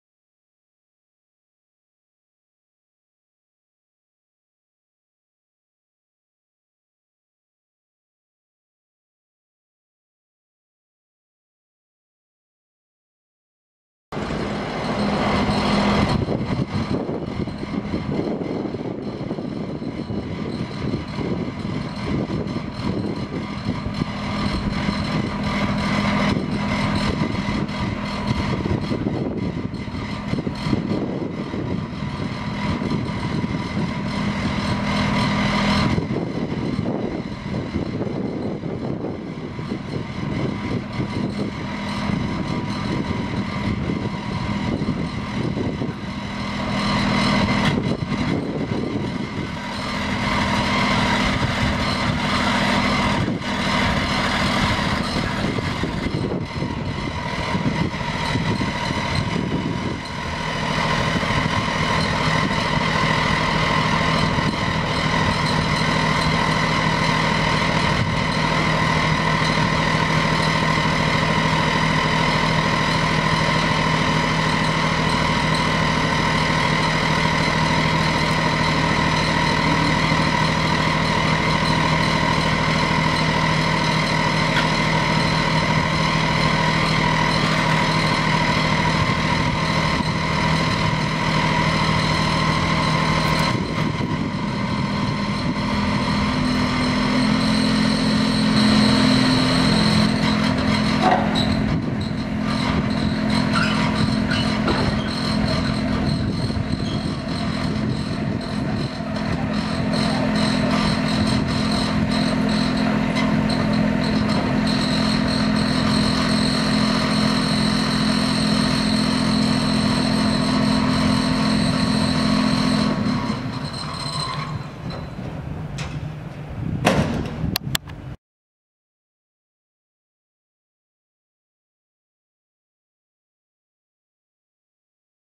Wacker LTC4L 2006
This second handed Wacker - LTC4L is in a good working condition and could be used directly. Pfeifer Heavy Machinery cares about organizing transportation, all necessary documents and everything related to the process of buying a used machine.